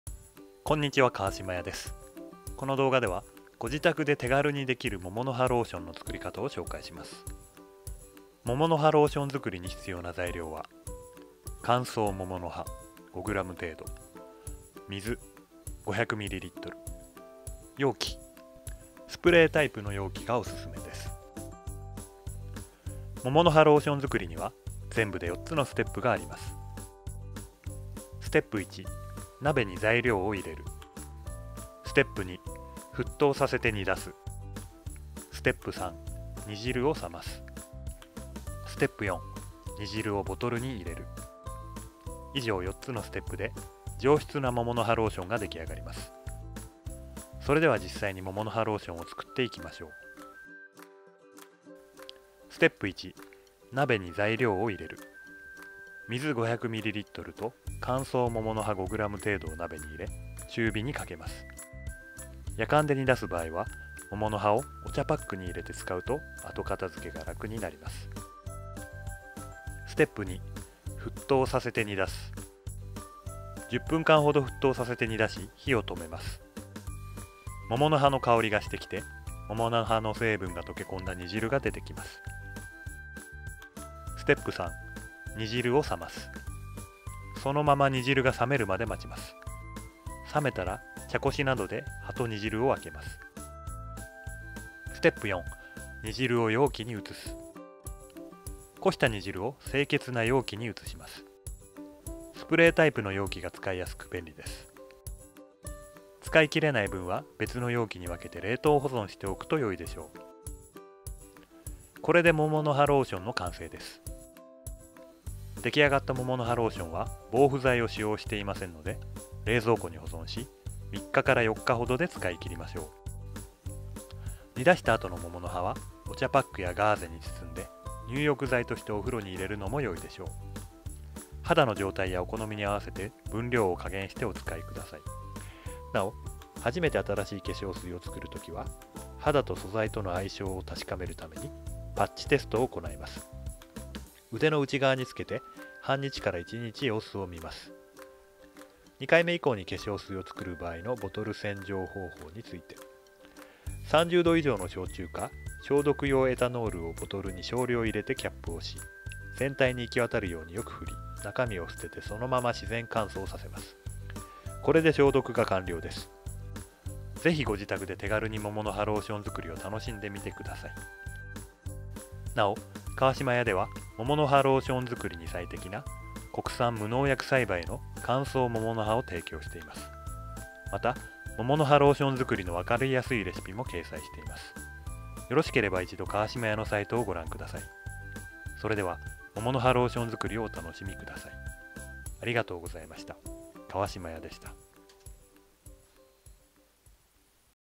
桃の葉ローションの作り方
乾燥桃の葉を使った、桃の葉ローションの作り方を解説する動画です。